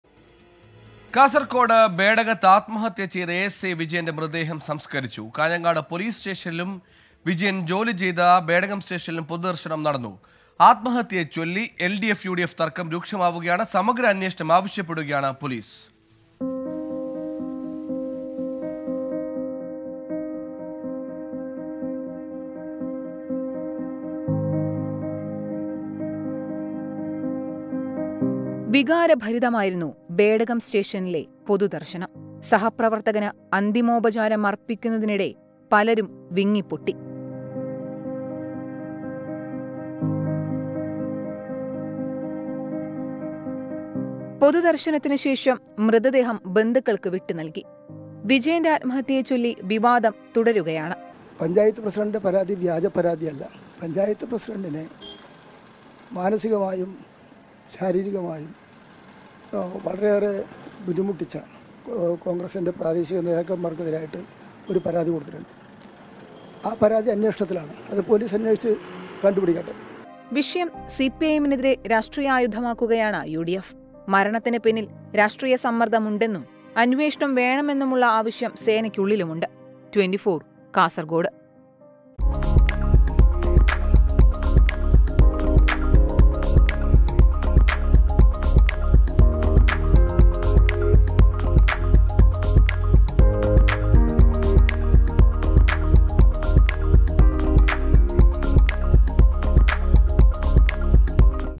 ബേഡകത്ത് ആത്മഹത്യ ചെയ്ത ASIയുടെ മൃതദേഹം സംസ്‌കരിച്ചു
ബേഡകത്ത് ആത്മഹത്യ ചെയ്ത ASIയുടെ മൃതദേഹം സംസ്‌കരിച്ചു; വിഷയത്തില്‍ രാഷ്ട്രീയ പോര് മുറുകുന്നു

Kasargod, ASI death